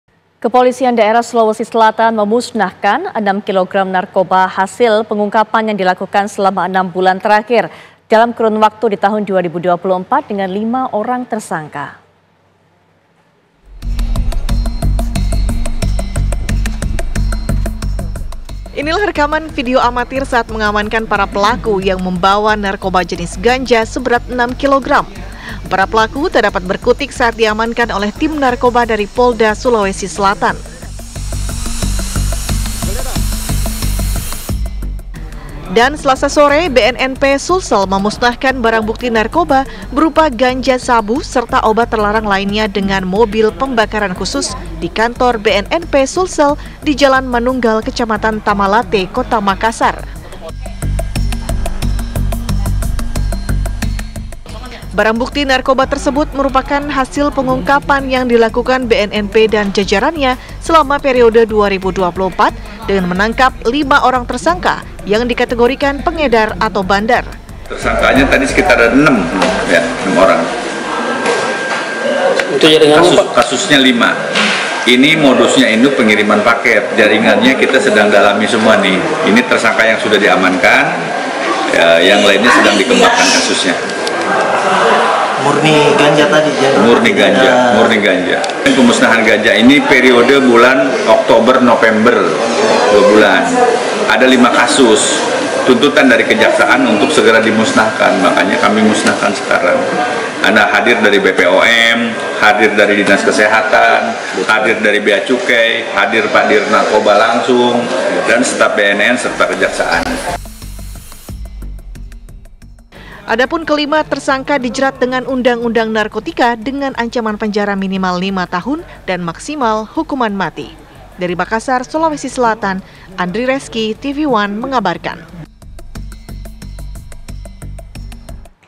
Polisi Bekuk Lima Pengedar Narkoba di Makassar, Sulawesi Selatan
Jakarta, - Polisi Bekuk Lima Pengedar Narkoba di Makassar, Sulawesi Selatan

KABUT01
FR01
FM01